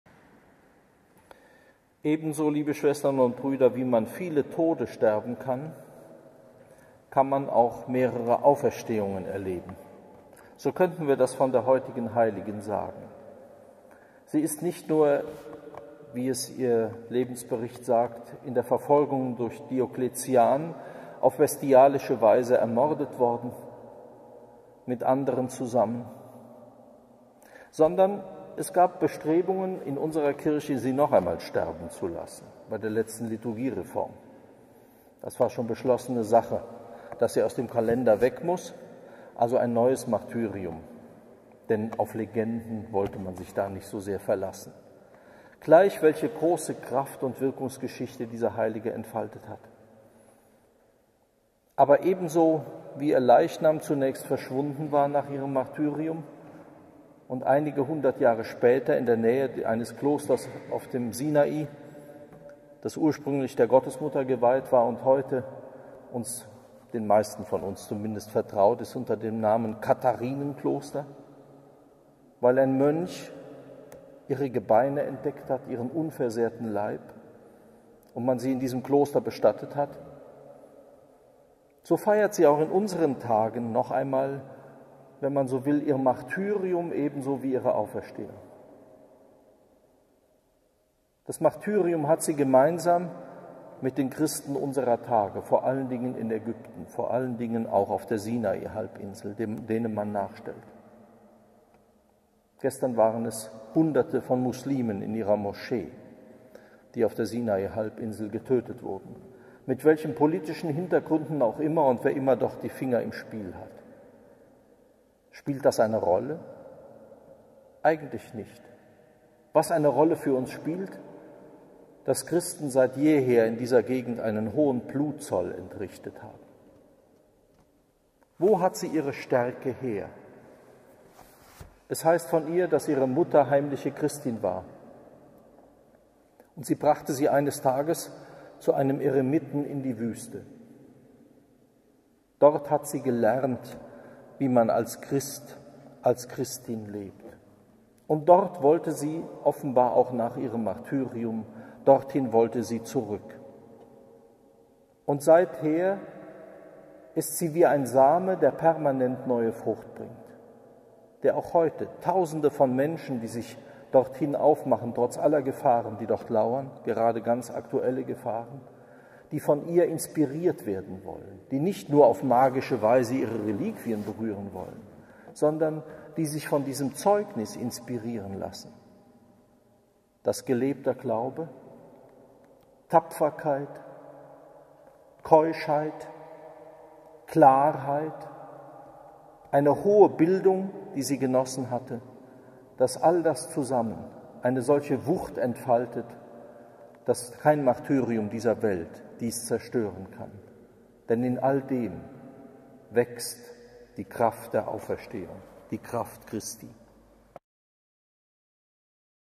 Ebenso wie es viele Tode gibt...
Predigt von Rektor Dr.Johannes J.Kreier am Gedenktag der Hl. Katharina im Kloster am Rande der Stadt/ Saarbrücken 2017.